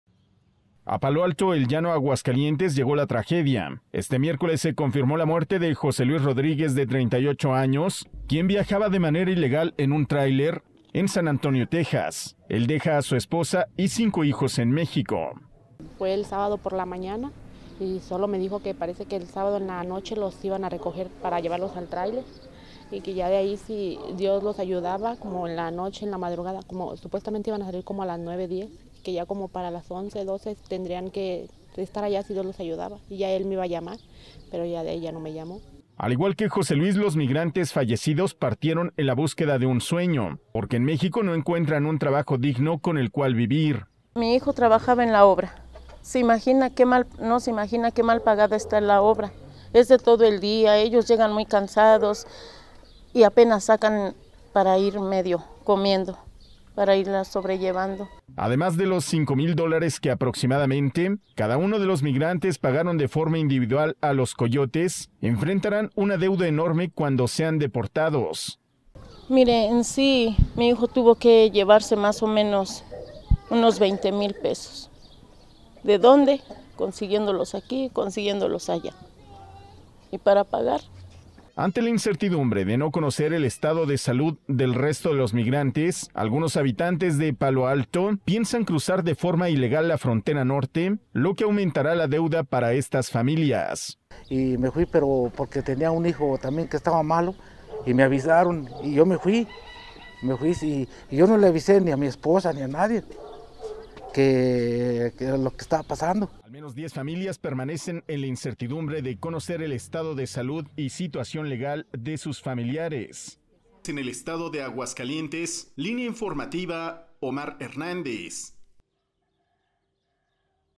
AGS: LLEGA LA TRAGEDIA AL LLANO AGUASCALIENTES
liin.tv ::LLEGA LA TRAGEDIA AL LLANO AGUASCALIENTES
Se confirma la muerte de José Luis Rodríguez, migrante que viajaba en un tráiler de manera ilegal en San Antonio Texas